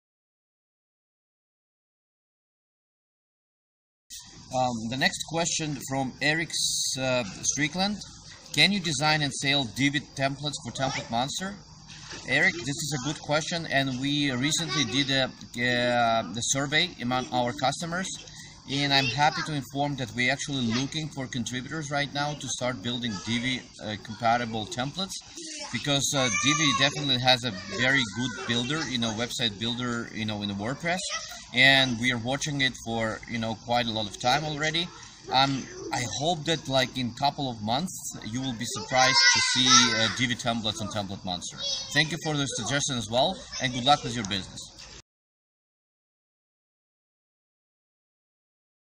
Ask the Monster: Can you design & sale Divi web templates for TemplateMonster? (Eric Strickland)
Please welcome David Braun, CEO & co-founder TemplateMonster.
He is ready to provide you with all the answers in this video playlist!